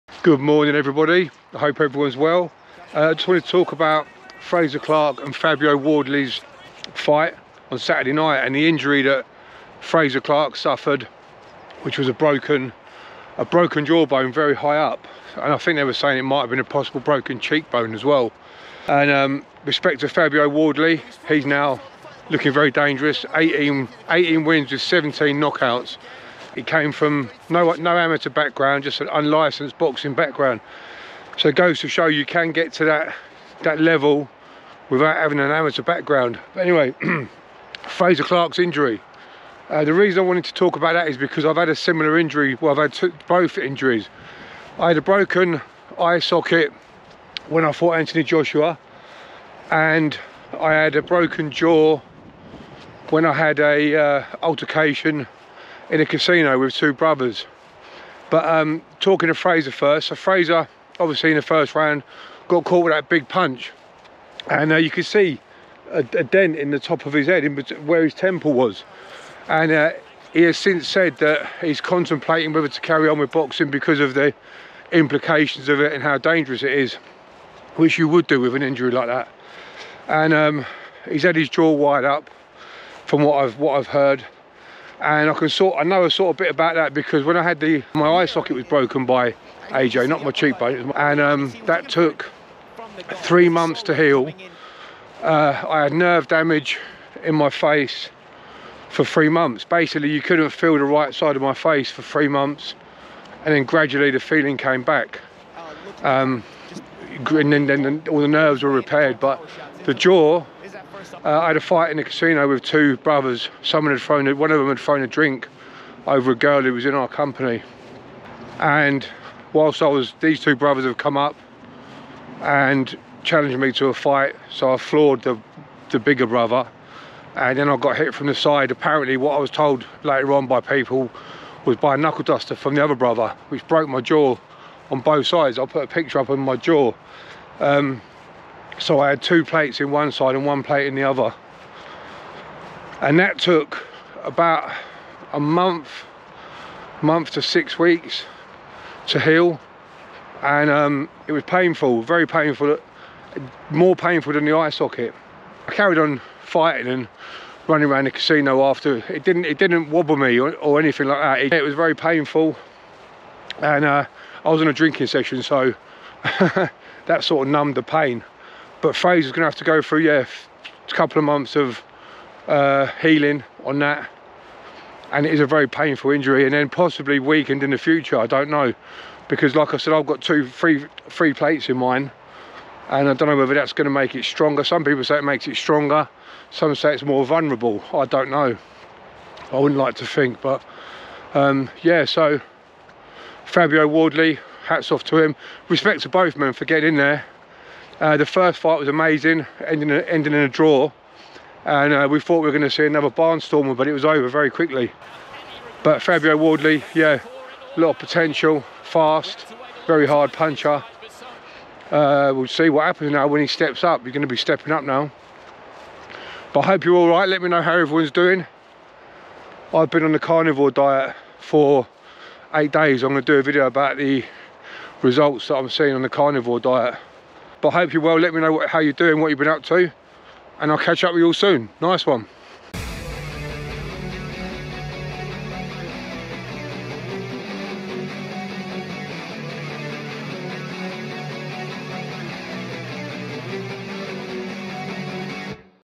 Frazer Clarke's BRUTAL JAW INJURY! MY experience of a BROKEN JAW/EYE SOCKET & Fabio Wardley Rising!
Fabio Wardleys 1st Round Stoppage of Frazer Clarke, left Frazer with a serious injury to his Jaw.